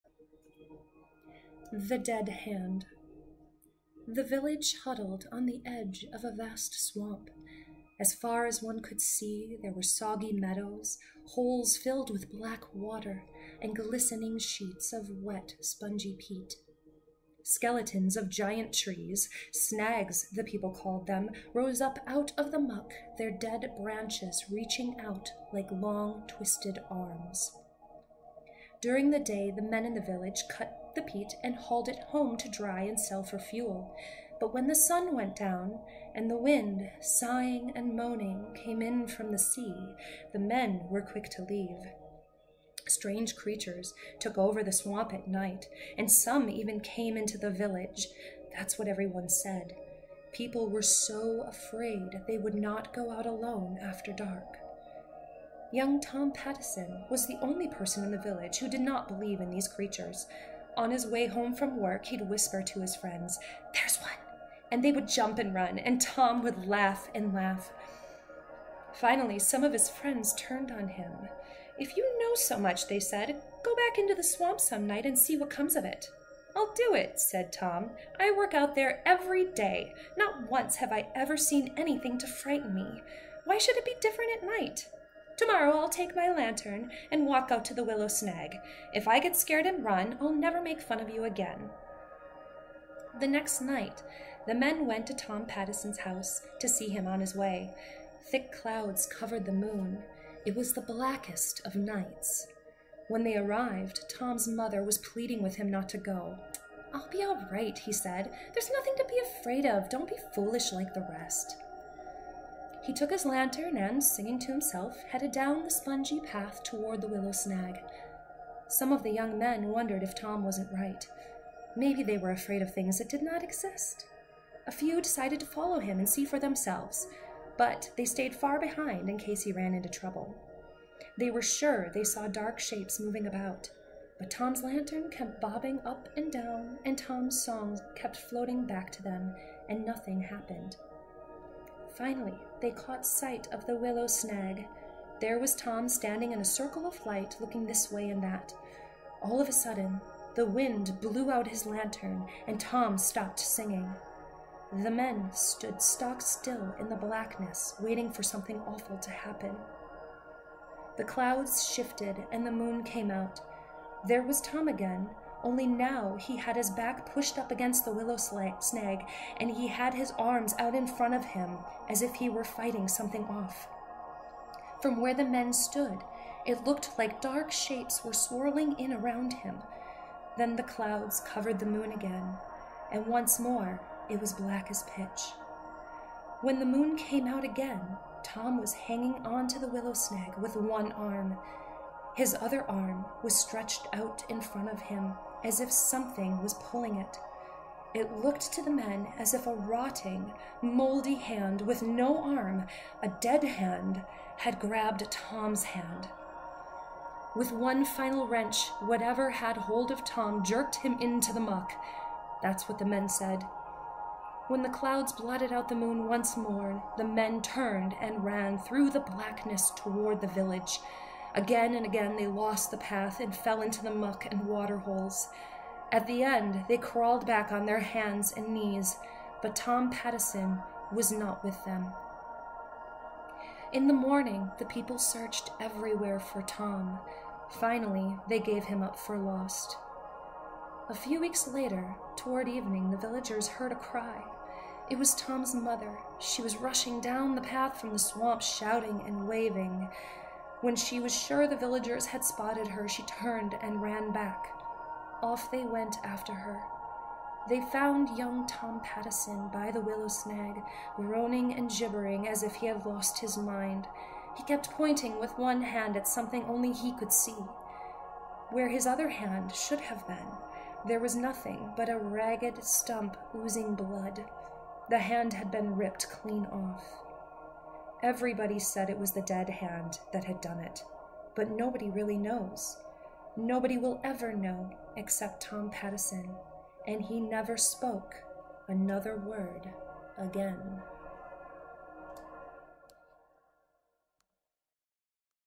"The Dead Hand" from Scary Stories to Tell in the Dark 3, read by Melissa Ferlaak
I dont know about you, but I have loved this treasury of spooky stories since I was a child,..the illustrations by Stephen Gammell are mesmerizing! As a mom (Melissa here, of course), I'm always reading stories aloud to my kids, so I thought I'd do the same for all of you! This one is called, "The Dead Hand" from Scary Stories to Tell in the Dark 3, collected from folklore and retold by Alvin Schwartz with Illustrations by Stephen Gammell.